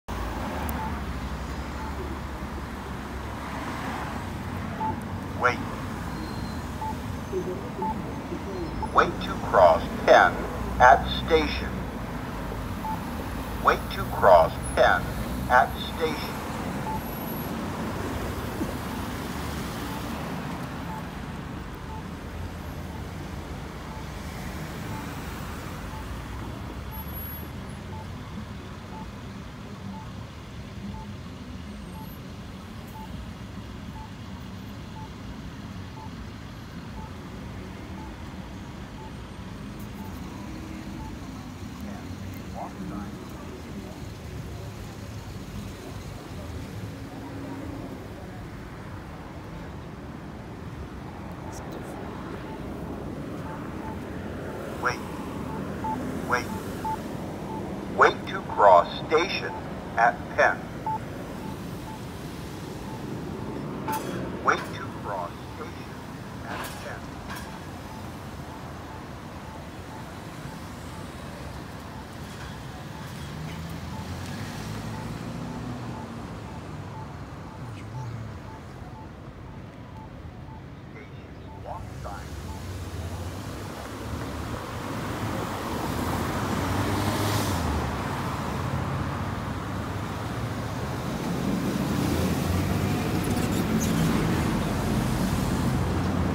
Campbell AGPS - Penn Ave. & "Station" | Larimer, Pittsburgh , PA
[Recorded: 2/3/2020]
This is a mast arm intersection from the 1980s when the shopping center was built. Everything except the poles were replaced over 10 years ago, then they added a crossing on one side, added ped-heads there and replaced the visors on the existing ped-heads, and added Campbell Advisor Guide buttons.

SIGNAL INFO:
8" heads: N/A.
12" heads: Econolite Aluminum and McCain (aluminum; originally T/Con).
Pedestrian heads: ICC/IDC (Peek) (aluminum) 16" with Dialight LEDs (+1 with Leotek LEDs) and McCain (aluminum) 16" with GE LEDs (originally T/Con).

PLEASE NOTE: If anyone's face appears in the video and would like to be blurred-out, please let me know, and I'll try my best to do so.